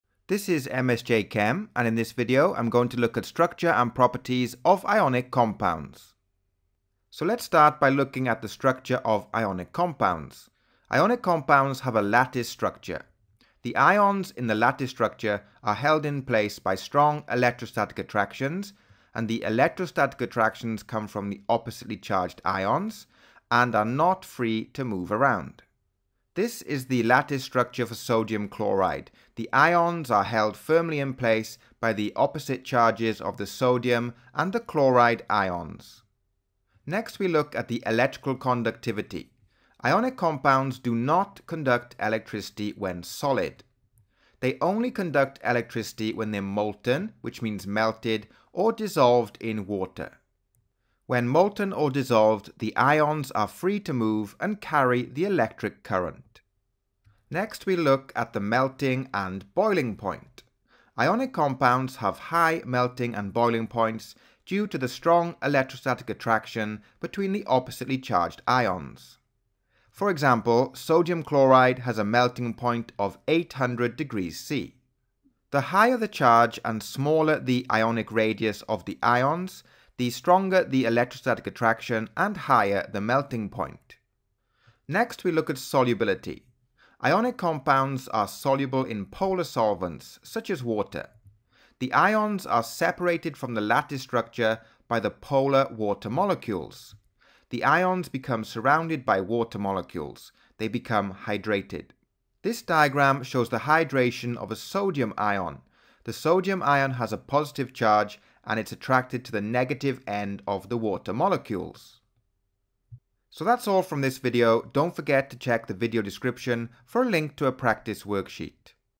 4.1 Structure and properties of ionic compounds (SL)
This video covers the structure and properties of ionic compounds.